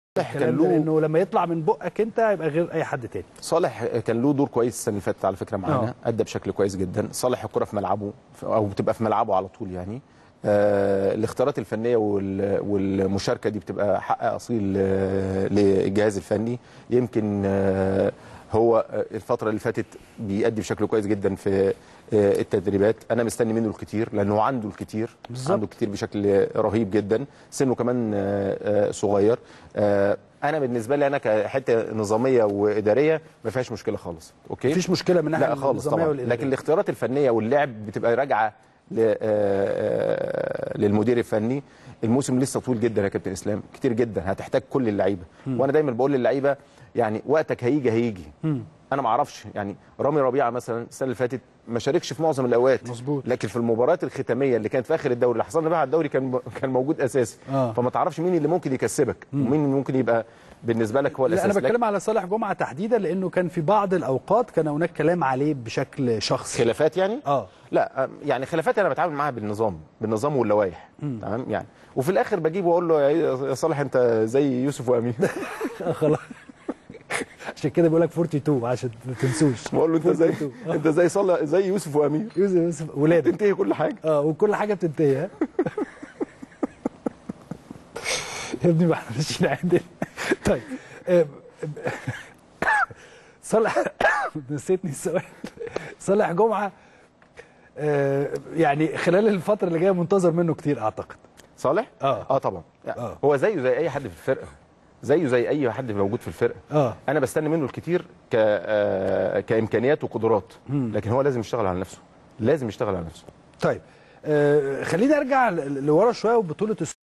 سيد عبد الحفيظ : "صالح جمعه" ملتزم وليس هناك اى مشكلة ومشاركته امر فنى
التردد: 12341 أفقي.
القمر: نايل سات.
معامل الخطأ: 5/6.
معدل الترميز: 27500